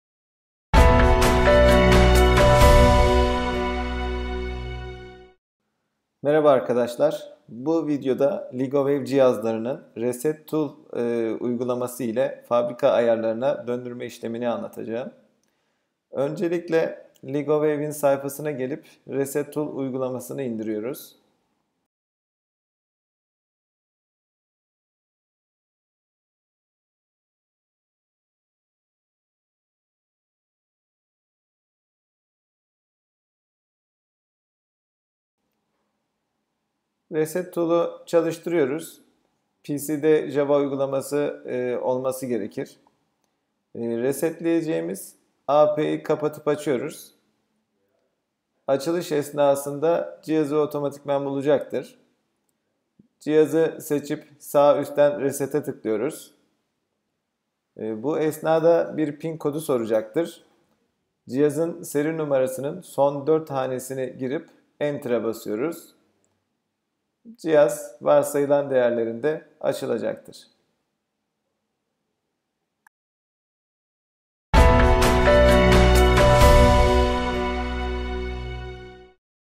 LigoWave Reset Tool Kullanımı
LigoWave Reset Tool Uygulması ile AP 'leri Fabrika Ayarlarına Döndürme